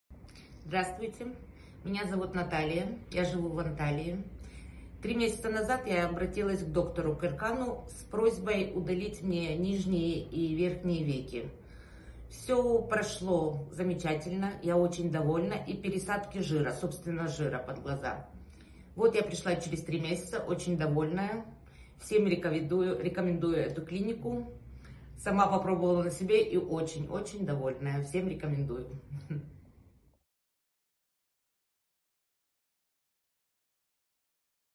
Vorher & Nachher || Augenlidchirurgie und Blepharoplastik in der Türkei Bewertung
Hallo, mein Name ist Natalya. Ich hatte vor 3 Monaten eine Ober- und Unterlidstraffung bei Dr.Erkan… alles ist reibungslos gelaufen. Bin mit dem Ergebnis sehr sehr zufrieden. Hatte auch eine Fett Injektion womit ich sehr glücklich bin…
Nach 3 Monaten bin ich wieder hier, immer noch glücklich… ich empfehle jedem die solch eine Operation haben möchten, diese Klinik,in der ich die besten Erfahrungen mit hatte…

Informationen Bekommen: